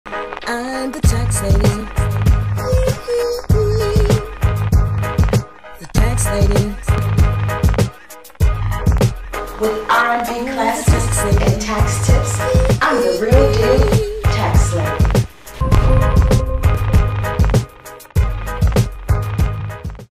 Hire an independent tax professional to file an accurate tax return.
A designated tax professional is here to help you file your 1040 tax return this tax season. We have filed thousands of tax returns for individuals with W2s as well as for independent contractors and small business owners with 1099s. With remote tax preparation, we can file your return no matter where you live. Visit singingtaxlady.com for more information.